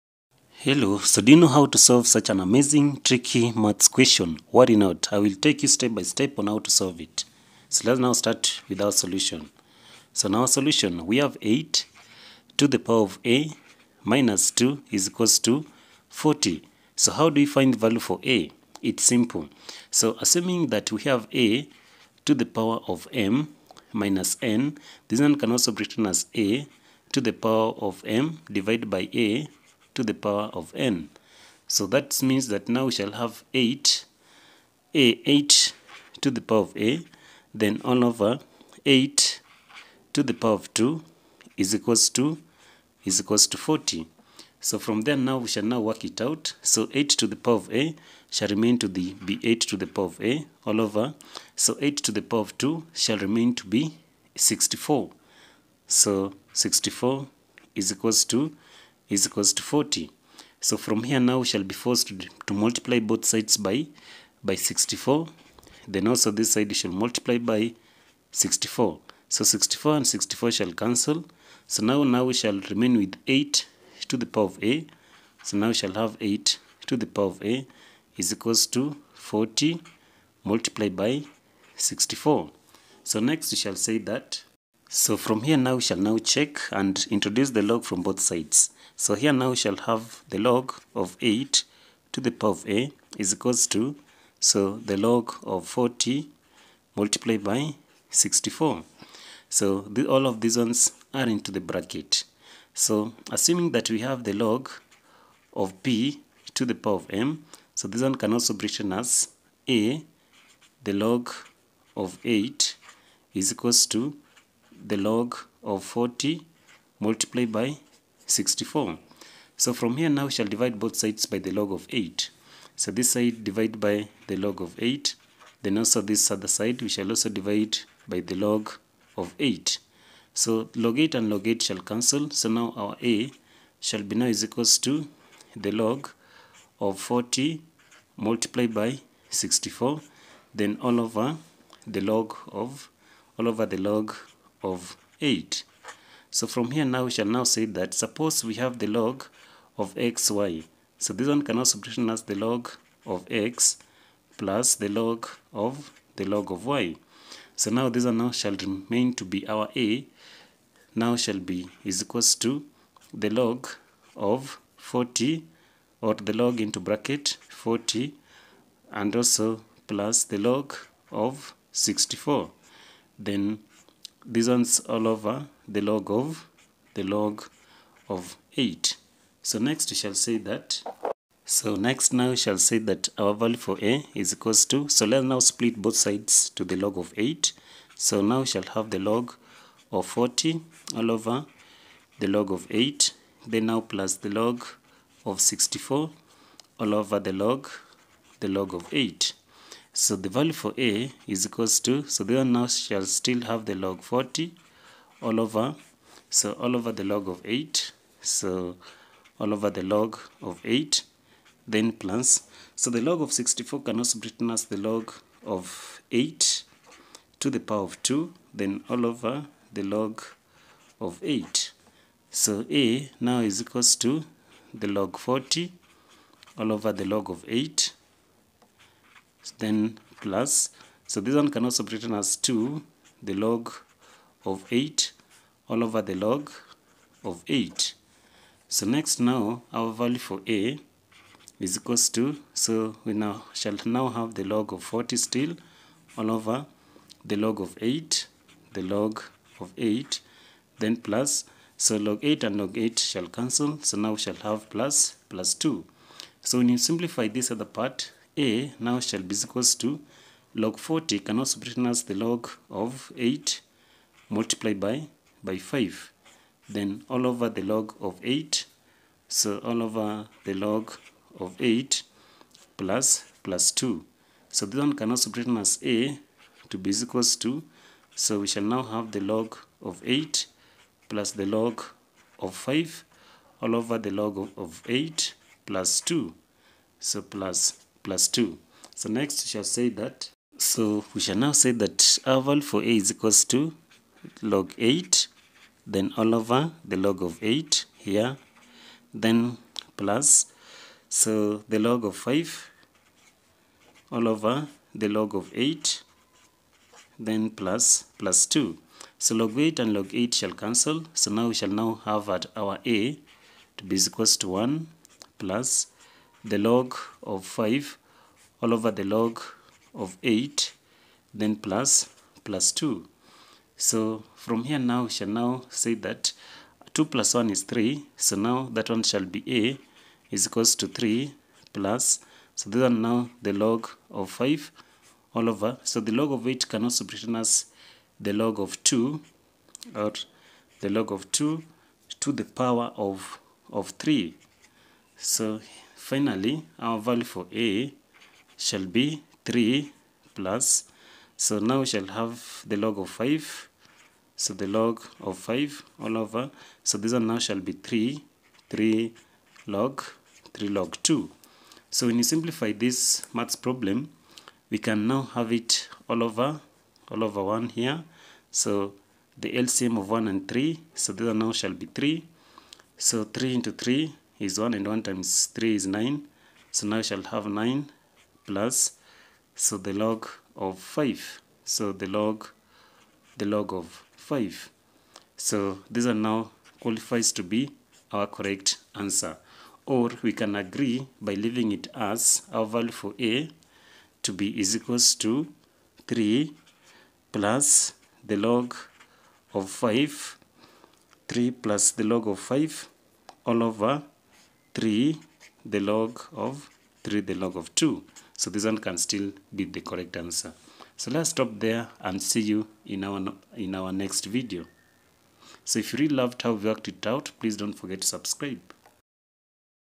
How to Solve this Tricky Math Exponential Equation Fast? | Olympiad Mathematics
In this short lesson, you'll learn how to: ✅ Solve exponential equations step-by-step
✅ Understand the logic behind exponents
✅ Build your problem-solving skills for math competitions & exams

Perfect for:

Math Olympiad students

Competitive exam prep (SAT, KCSE, IGCSE, etc.)

High school & college math lovers

💬 Drop your solution in the comments and see how you rank!